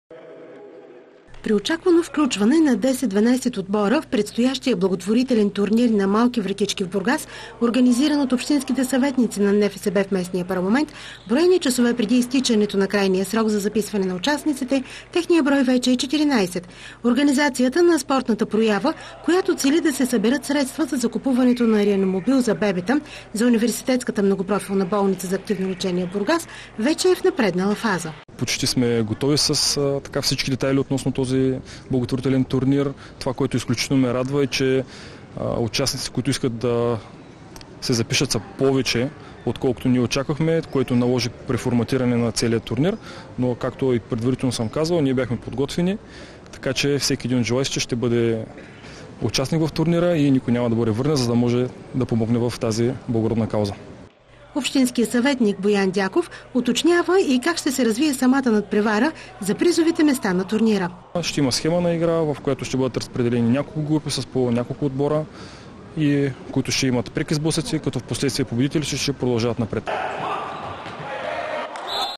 Голям интерес към благотворителния футболен турнир
В последния ден от срока за записване на участниците в благотворителния футболен турнир, който ще се проведе на 15 декември под егидата на общинските съветници на НФСБ в Бургас, организаторите отчитат изключително висок интерес към мероприятието